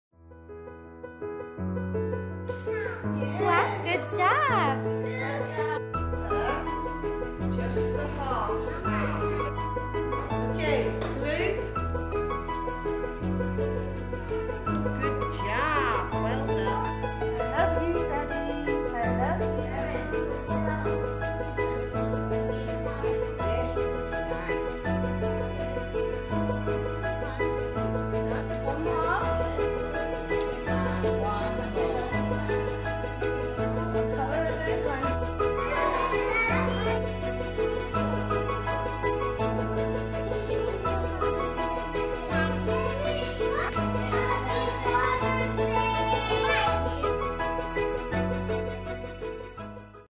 Father's Day
Today at Blossom Marina Nursery in Dubai, our Tulips class used their imagination to create a lovely card for their Dads.  For all the good times, hugs and kisses we shared and for holding your hand... we wish you a Happy Father's Day!